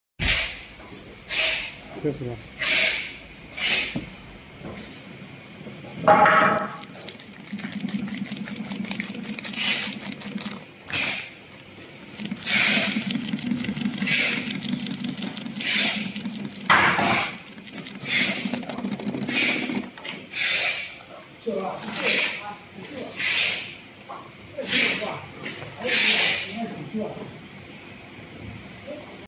Woven Sack Sewing Machine At Best Price
HighTex  FIBC800 Woven Sack Sewing Machine At Best Price and durable quality
Heavy duty high speed chain stitch sewing machine with top and bottom feed mechanism, specially designed for heavy weight material such as PP, HDPE, Leno, Non Woven and Multiwall Paper and other types of woven sacks.